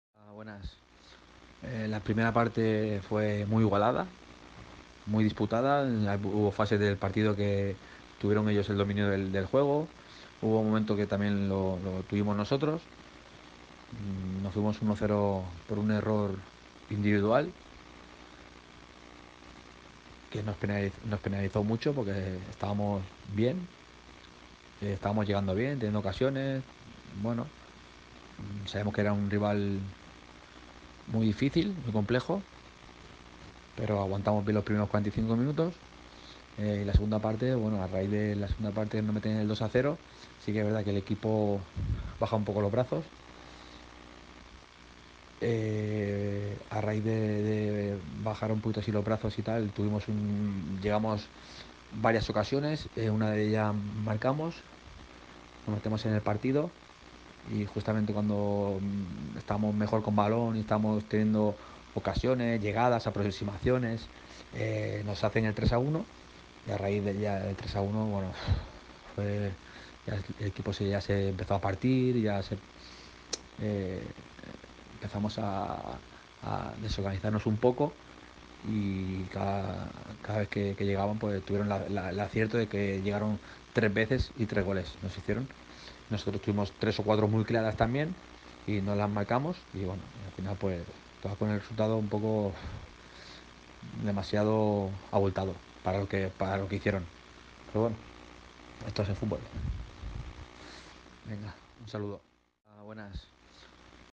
Declaraciones de Chava Jiménez, entrenador del FC Argentona (Jornada 22)
🎙Escuchamos a Chava Jimenez entrenador del @fcargentona tras el partido de la jornada 22 🧑⚽️😀👍maresme